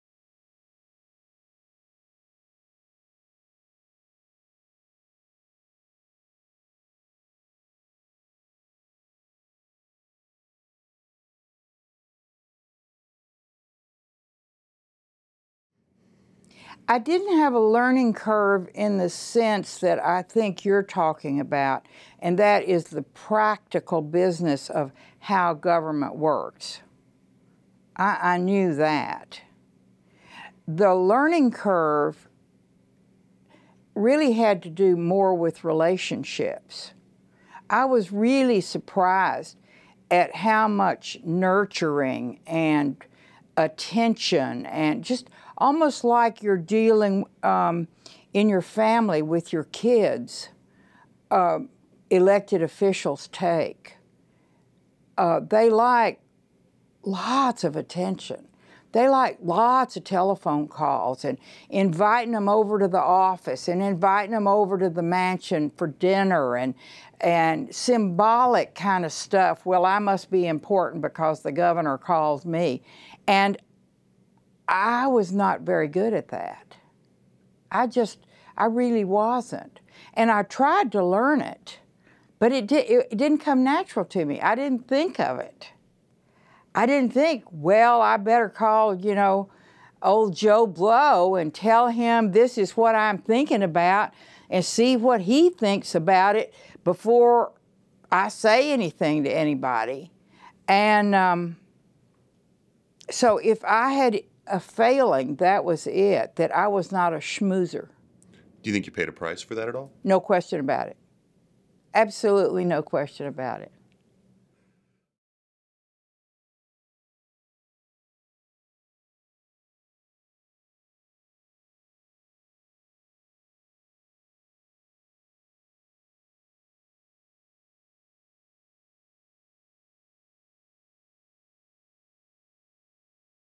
Former Governor Ann Richards Discusses Her Relationships with Other Politicians
Ann Richards was asked what she had to learn when she became Governor.  From a 2003 interview conducted for the Texas Politics project.